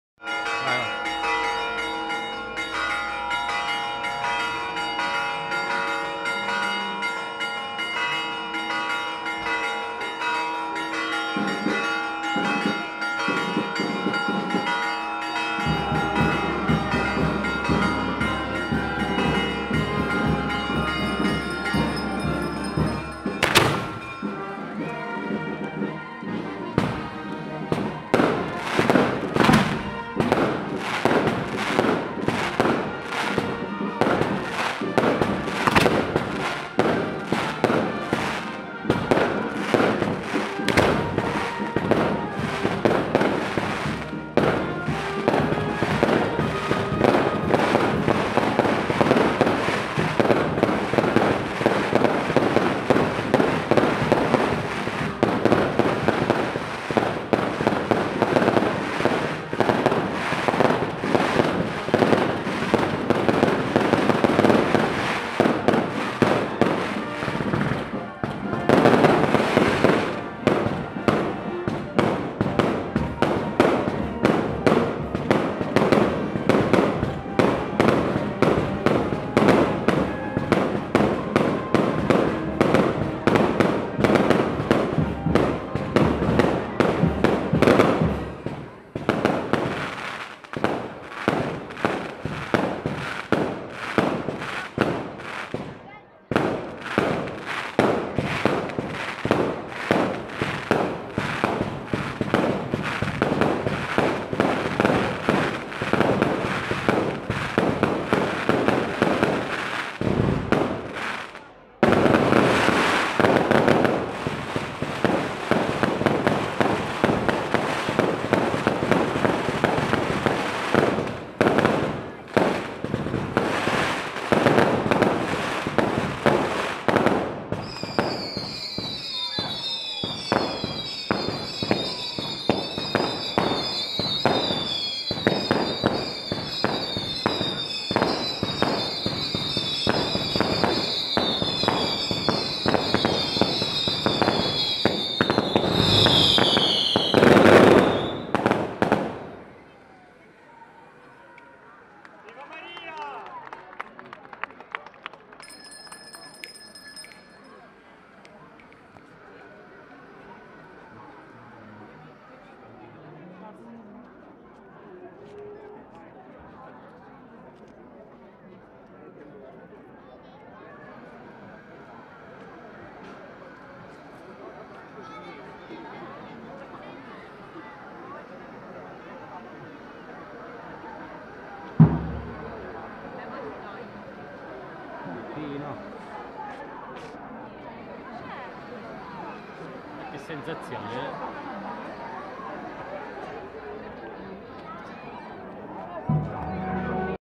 S.GIOVANNI LA PUNTA,8 SETTEMBRE 2018 - FESTA MADONNA DELLA NEVE WP 20180908 185
Festosa uscita della Madonna con i fuochi curati dalla ditta "NUOVA PIROTECNICA" di Nicola Occhino da Santa Venerina